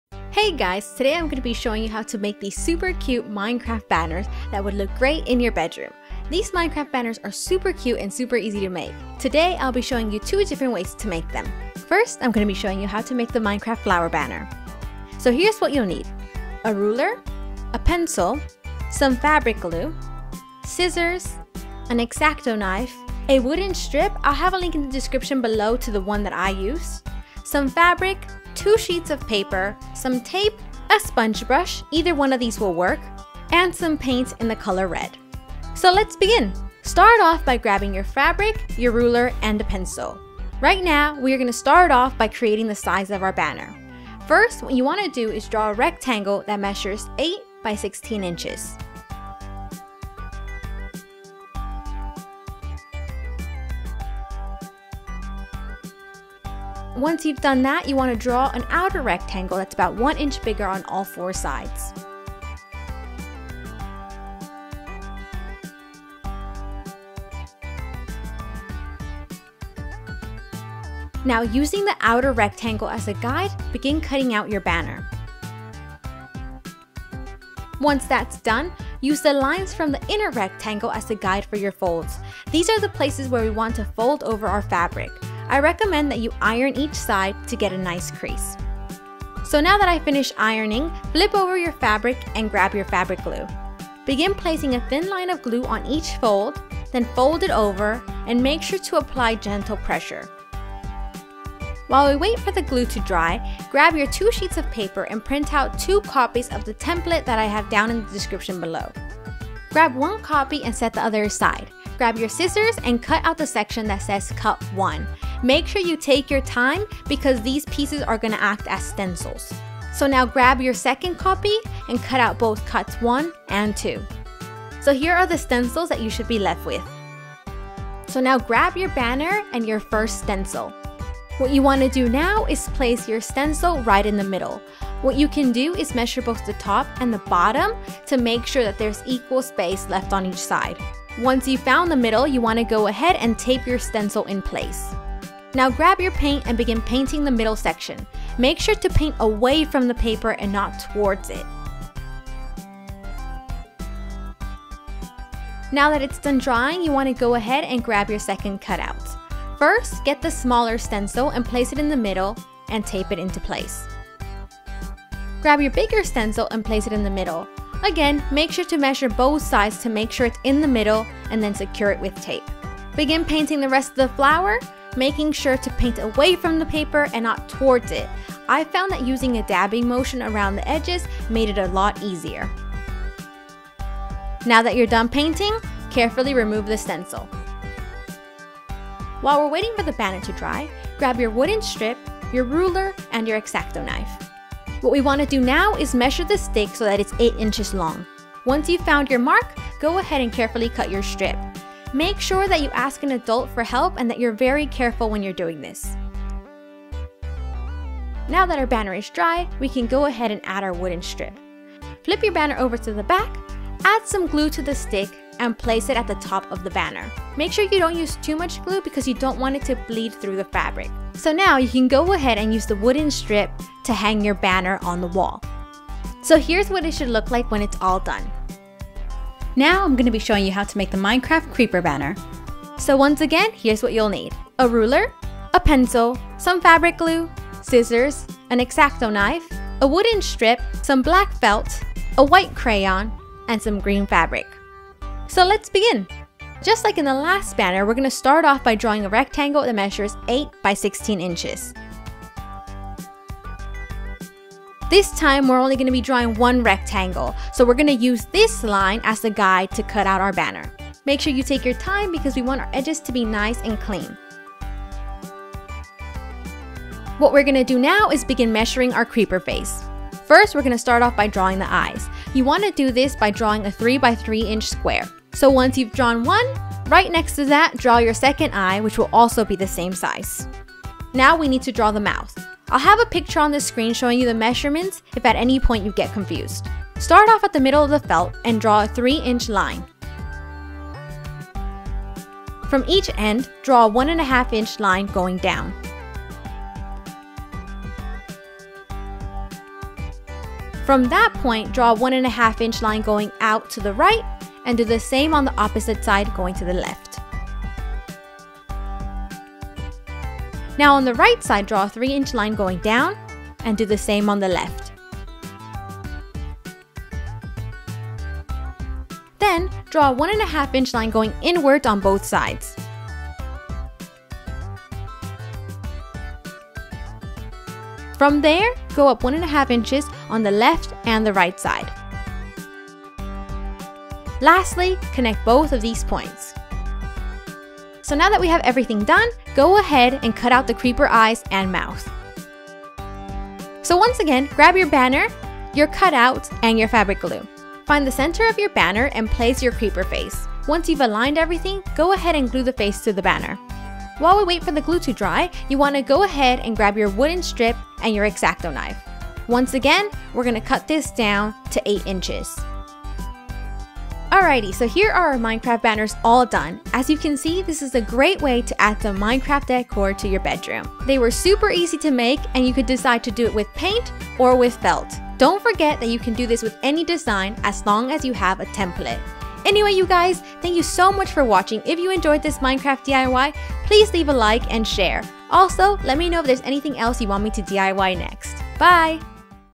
Minecraft: Banner (DIY)
Minecraft Banner DIY! Today I’ll be showing you how to make a Minecraft DIY pf a Minecraft Creeper Banner and a Minecraft Flower Banner  to bring you Minecraft in real life.  It’s a cute and easy Minecraft DIY craft that will add a touch of realistic Minecraft to your life!

In this Minecraft DIY and we’ll be crafting two Minecraft Banner, a Minecratf Flower Banner and a Minecraft Creeper banner! The Minecraft banner can be used as Minecraft room décor or it can be used a Minecraft party decorations!

Items you will need for both Minecraft Banner:
●  Ruler
● Pencil
● Fabric Glue - $1 at Dollar Tree
● Scissors
●  Wooden Strip -$1.19 ay Michaels (Bosswood Strip 3/16 x 3)
Additional items for the Minecraft Flower Banner:
● Fabric – I’m using canvas fabric
● Two Sheets of Paper
● Sponge Brush - $1.99 at Michaels ( 8pc  Brush Sets)
● Red Paint – $0.79 at Michaels (Craft Smart)
● Minecraft Flower Banner Template:
Make sure to drag the image to your desktop before printing
Additional items for the Minecraft Creeper Banner:

If you read this leave a comment saying ► MinecraftDIY!

- ZaiLetsPlay